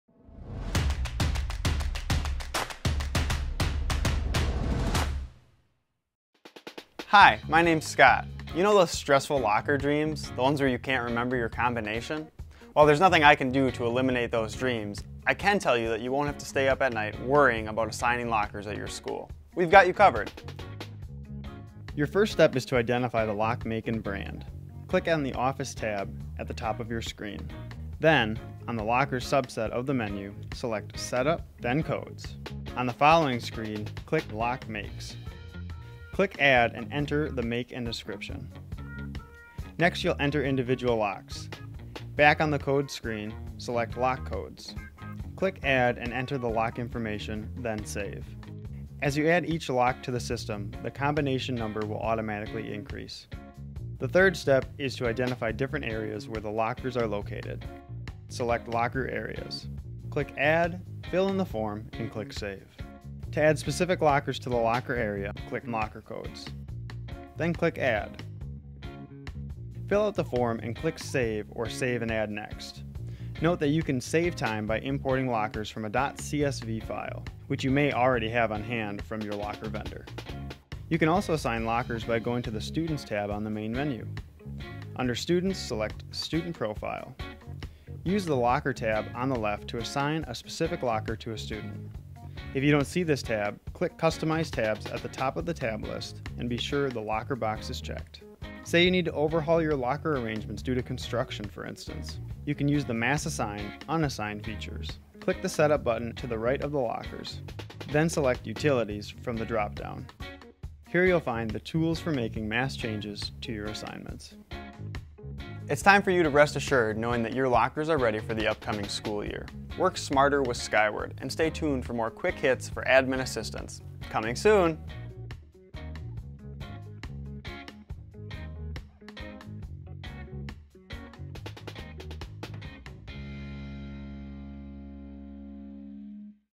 Quick Hits: Locker Assignments
Learn how to add, edit, assign, and mass assign lockers.